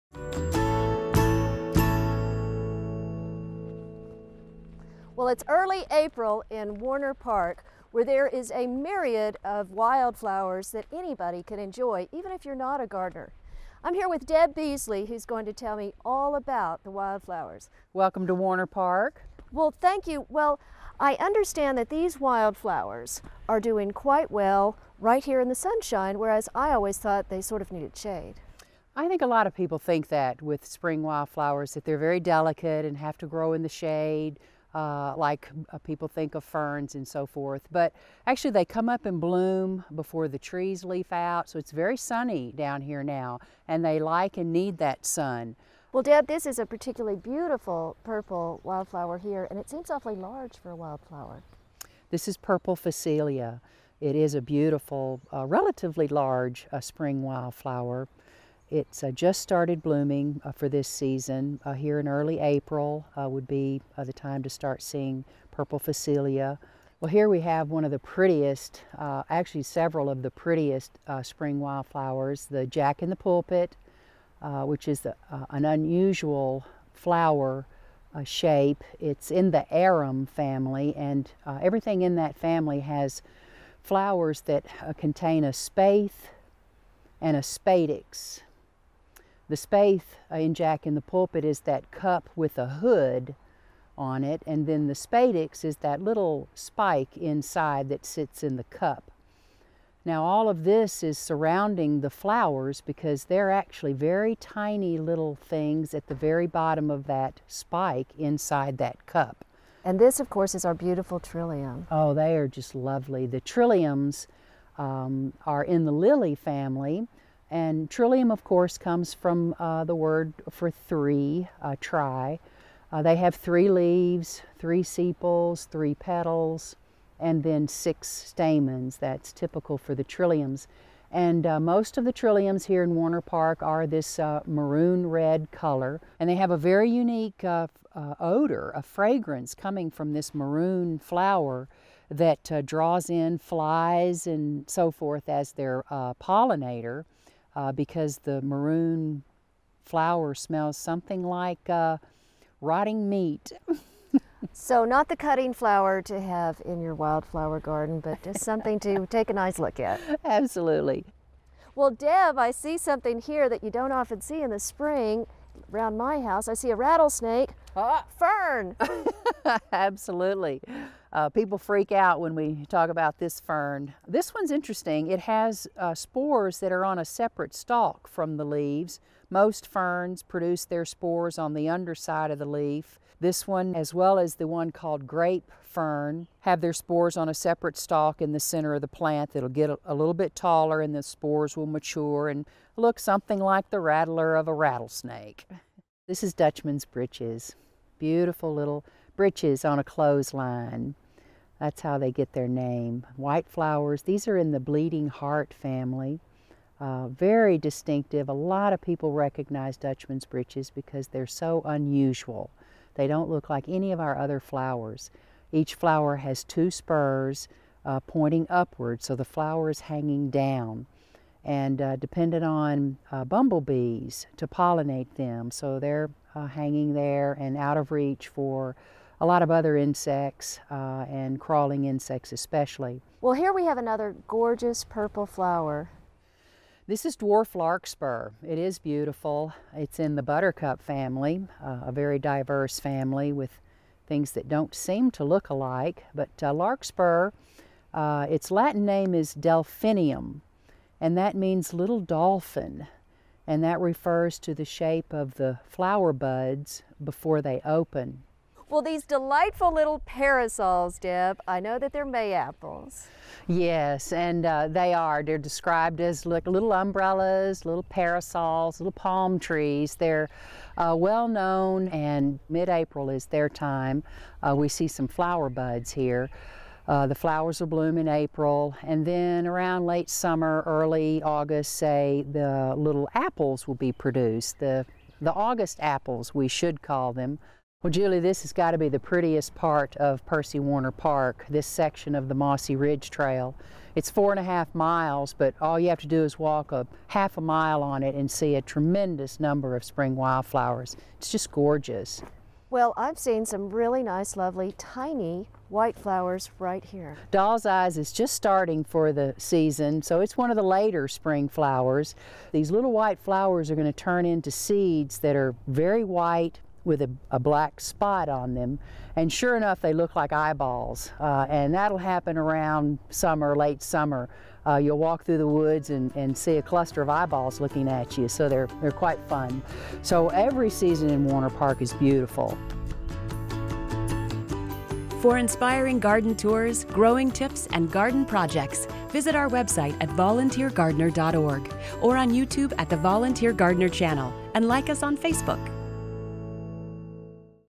Spring Wildflower Walk | Volunteer Gardener
Julie Berbiglia takes us on a spring wildflower walk.